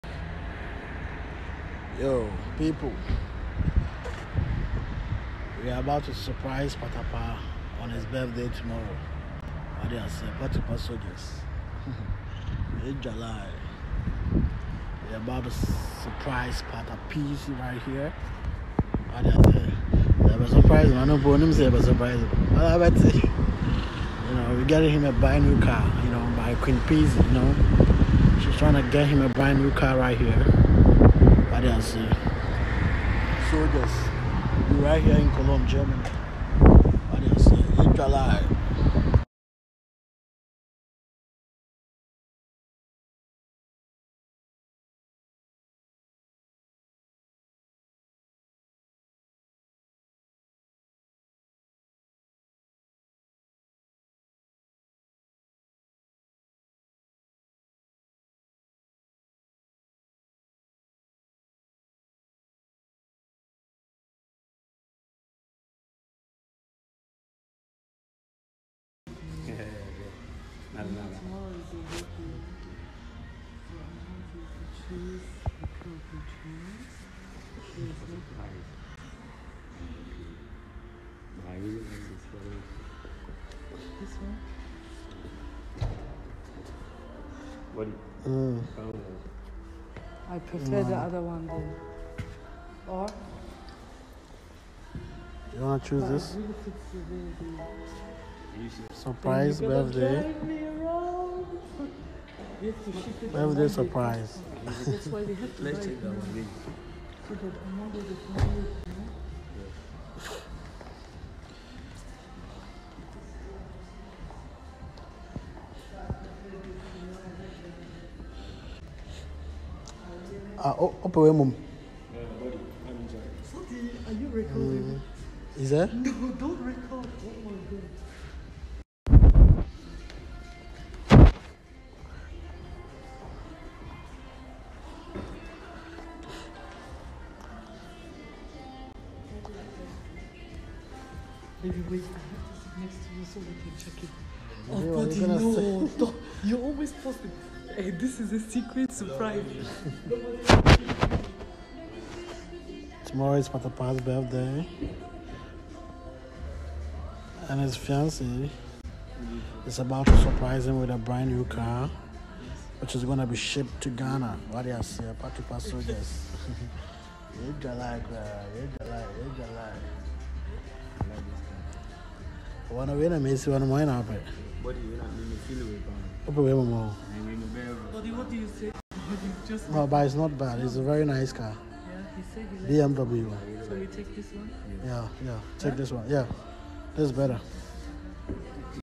Queen Peezy surprises Patapaa with a brand new Car ahead of his birthday
Queen Peezy Surprises Patapaa with a brand new Car ahead of his birthday. (C) 2020. Pa2Pa Soldiers

PLEASE NOTE: Unauthorized upload of this video unto any YouTube channel is prohibited. (C) 2020. Pa2Pa Soldiers